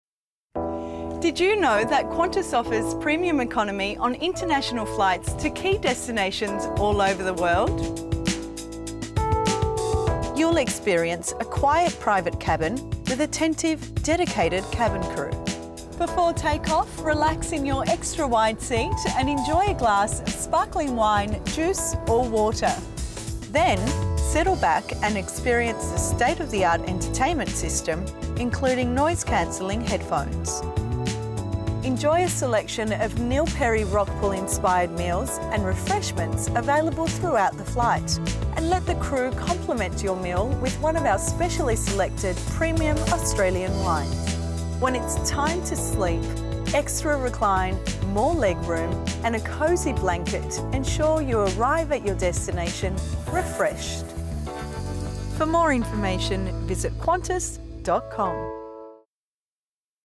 Qantas International Premium Economy
Find out all about the Qantas Premium Economy experience. Available on a number of international routes, enjoy that little bit of extra space and comfort next time you travel.